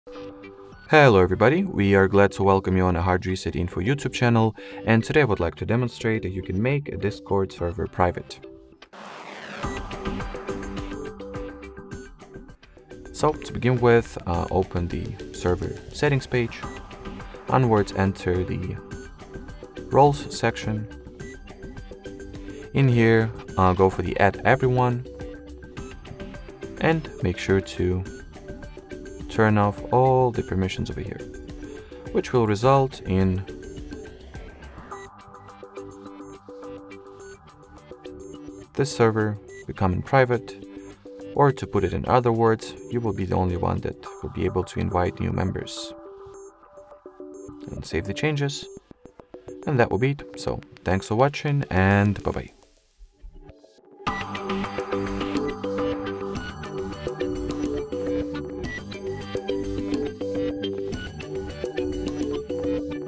How to Make a Discord Server Private on a Mobile App - Change the Discord Server to Invite Only
This video guide is going to serve as very detailed step by step instruction on how to restrict public access to your Discord server and make it, basically, an invite only place. Therefore, if you do not want for your server on a Discoird mobile app to be publicly and widely available, we humbly encourage you to view this tutorial.

How to restrict server access on Discord mobile app? How to change the Discord service privacy settings? How to make the Discord server invite only?